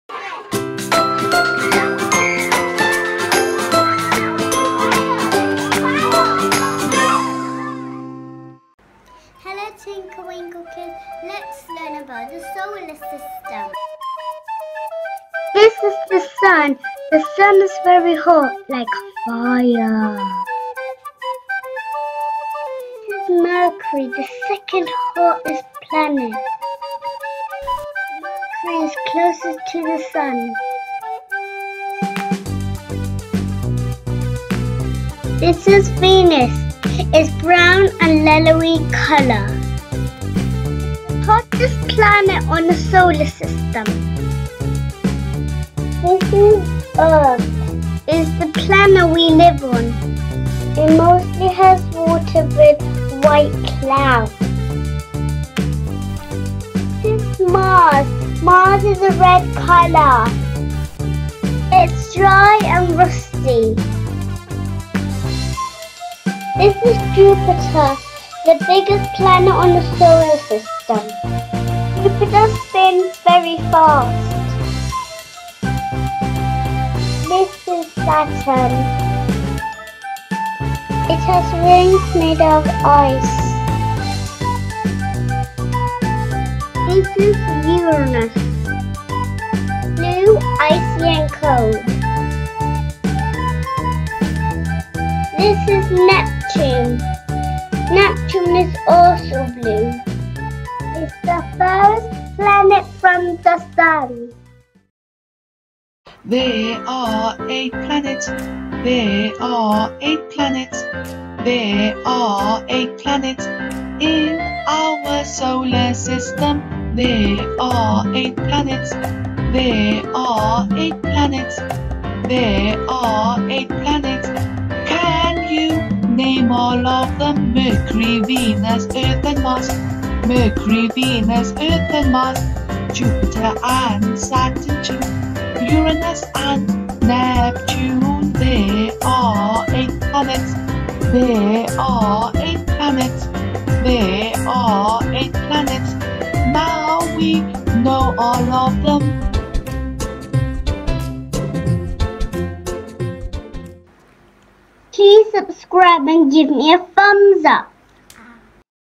Solar system | 8 planets | Solar system song | solar system for kids
Learn all about outer space from eight planets with Mercury, Venus, Earth, Mars, Saturn, Jupiter, Uranus and Neptune With this fun educational video made for kids! Fun, Educational, Enjoy!